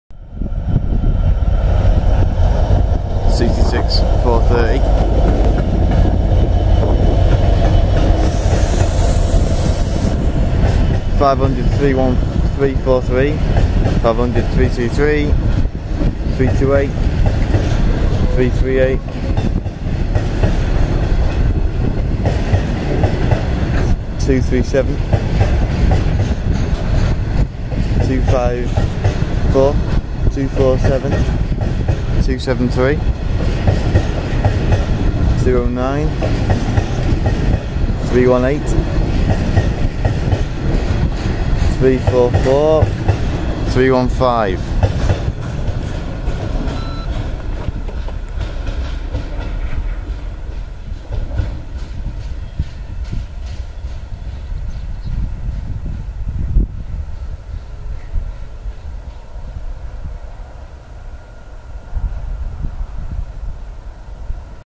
66430 working Carlisle to Mountsorrel
DRS 66430 working Carlisle to Mountsorrel at crewe Casey lane